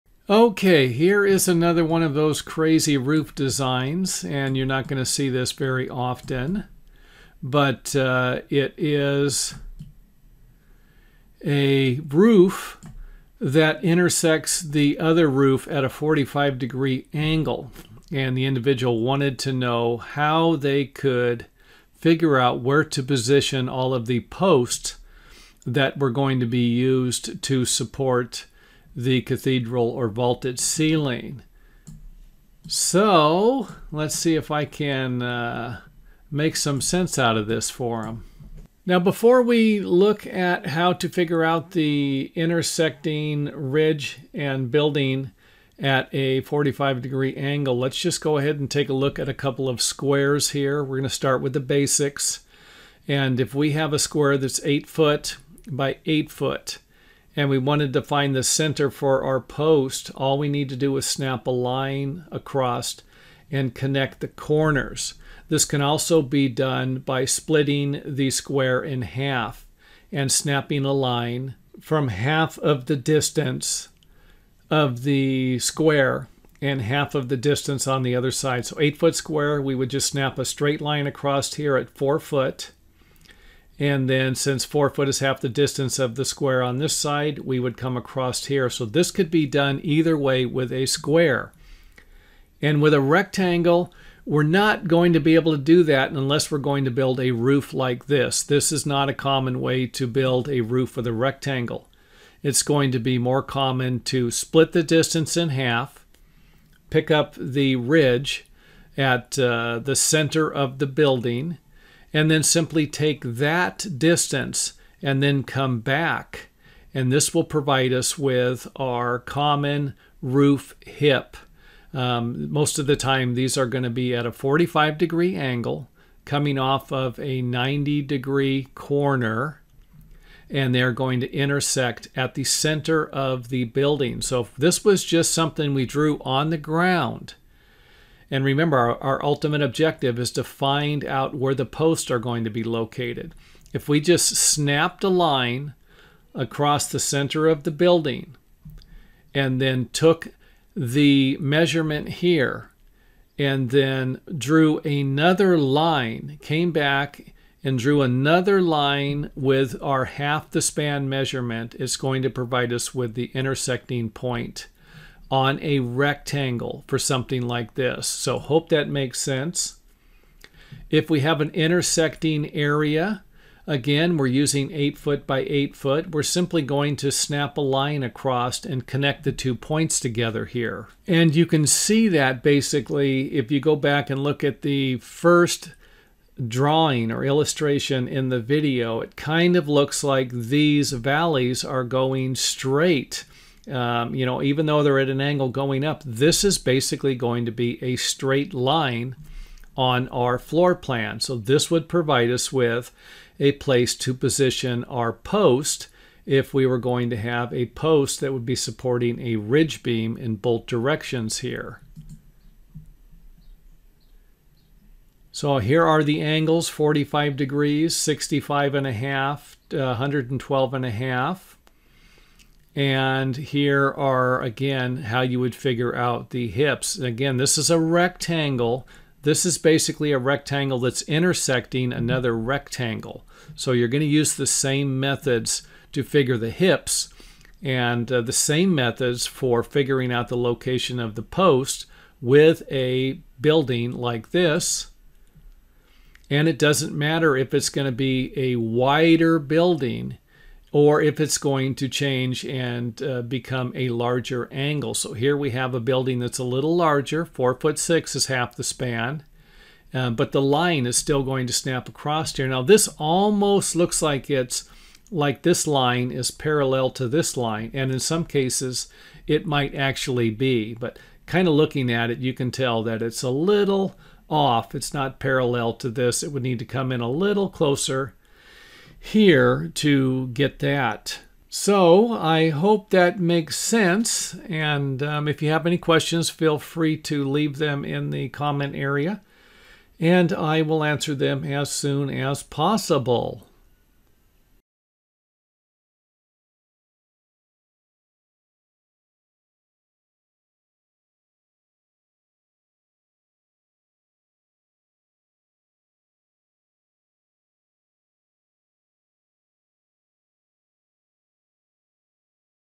How to Calculate Roof Ridge Post Location on Intersecting Angled Floor Plan - Home Building
Click on this link for more helpful videos about roof framing, building design and carpentry. This video will give you a simple method for figuring out how you can locate the exact area where a post can be located to support a ridge beam for a vaulted ceiling when you are dealing with sections of the building that are intersecting at an angle.